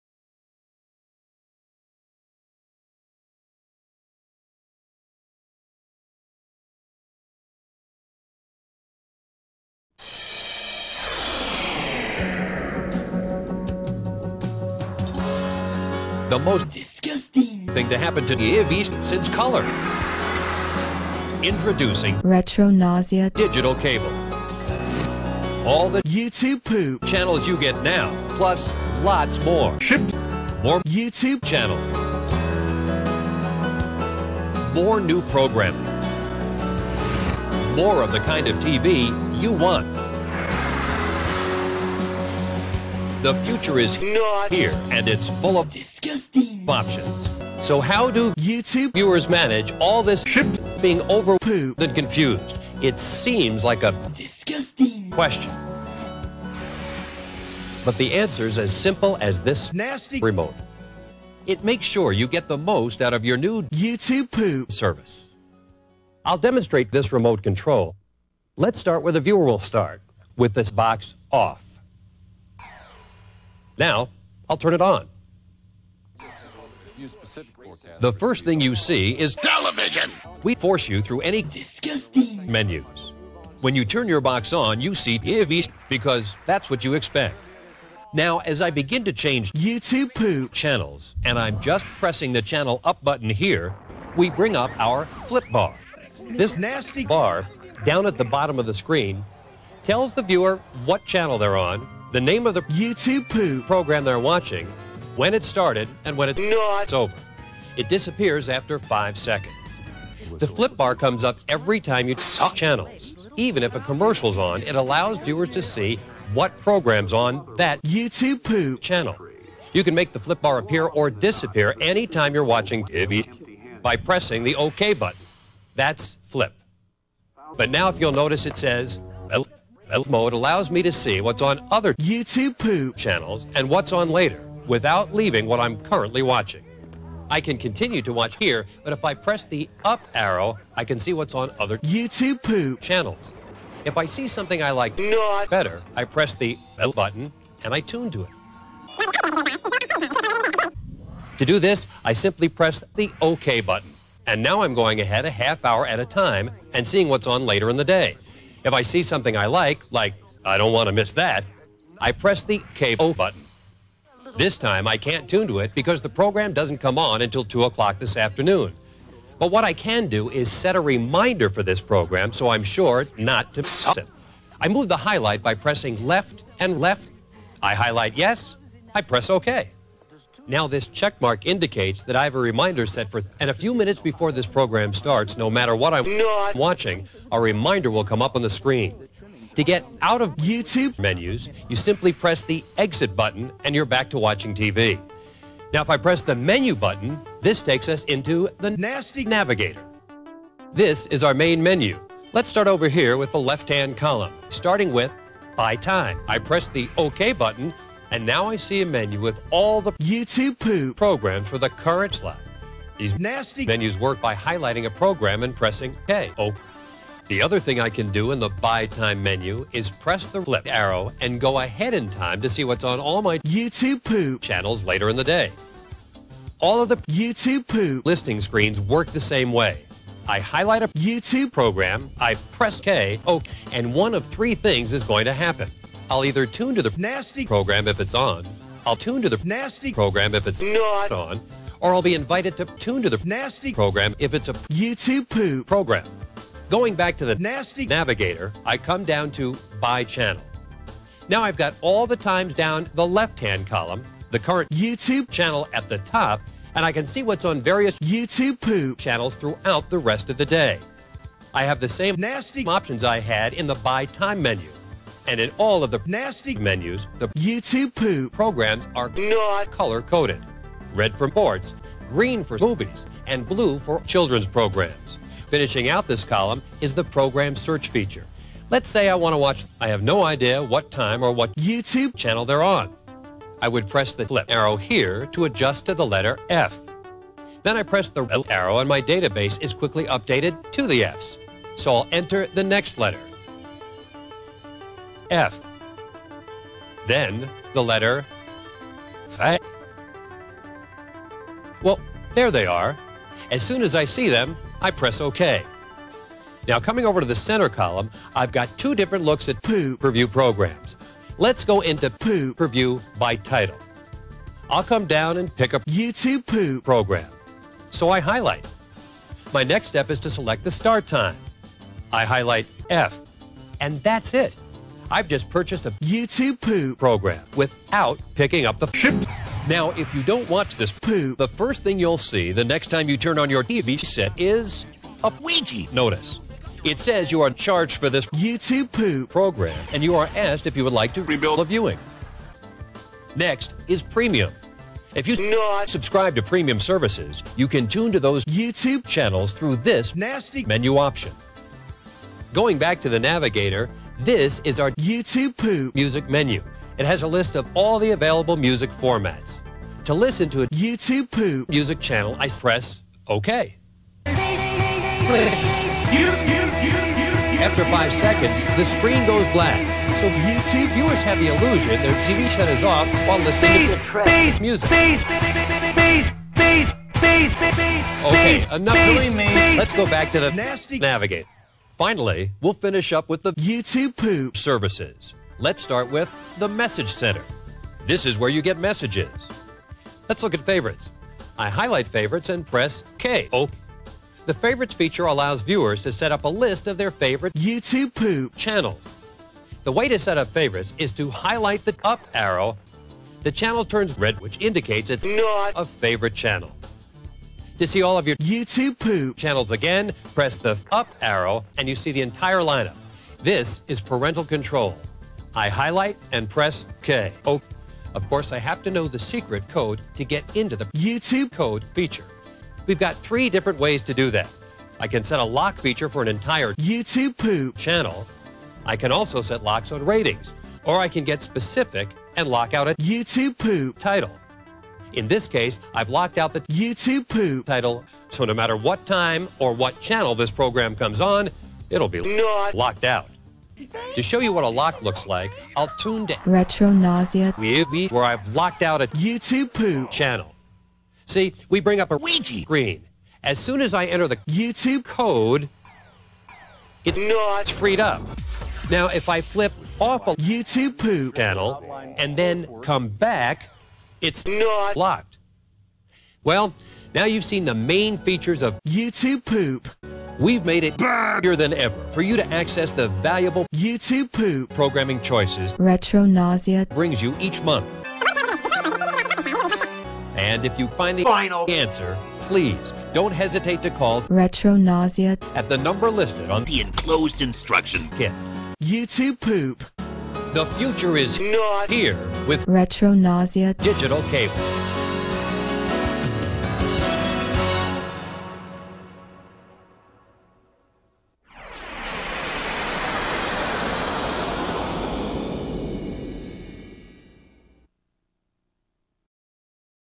An Innovative YTP/Digital Cable Instructional Video
This instructional YTP video lets you know about all the old and the new features that put on your emergency digital cable system.

Took me a long while to edit this, but hey, we thought this POOP may work for the nasty file to be more specific. Happy Thanksgiving to all of us!